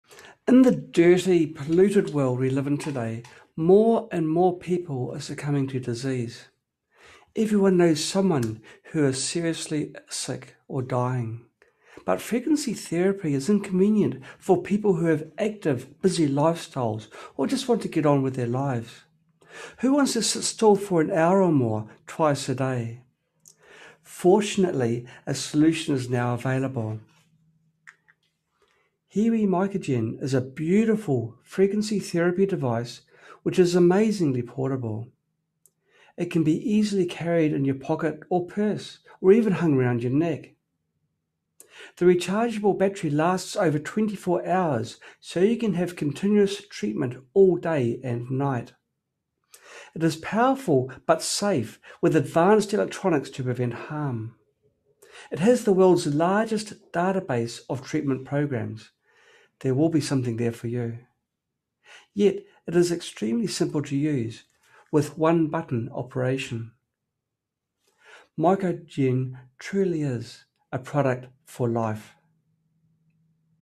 New Product Release: HeaWea MicroGen
HeaWea is our new brand, dedicated to creating leading-edge portable health products. HeaWea is an abbreviation of ‘Healing Wearables’.

MicroGen is the first product to be sold under the HeaWea brand. It is a portable therapy device like no other. It is currently in production and will be ready for sale on the 16th April.

So, what makes MicroGen different from all the other offerings on the market? Let’s take a quick peek at some of the features.

MicroGen can deliver both low-power and high-power healing frequencies.

Low-power (Frequency Specific Microcurrent) mode speeds up your body’s natural recovery processes. It can also help to relax muscles, relieve pain and stiffness.

High-Power mode delivers the punch to target serious health issues caused by viruses, bacteria, fungi, and parasites.

MicroGen has the power of much larger devices (up to 60 volts!), yet can easily fit into your pocket or purse, or hang from a convenient lanyard around your neck. A single charge lasts over 24 hours, and the battery can be charged from any USB port.

Choose programs from a massive database, which is regularly updated for free. The programs include the complete set of DNA frequencies for humans and animals.

MicroGen can hold 1,000 frequencies, with a resolution up to an incredible 0.000001 Hz. The maximum frequency is 2 MHz, with an unbelievable 40 nanosecond switching speed. (0.04 millionths of a second).

Using clever current-limiting circuitry, MicroGen can safely deliver Rife, FSM (Frequency Specific Microcurrent), Zapper and Blood Purifier treatments. And it is suitable for both humans and pets.

No other portable treatment device can match MicroGen.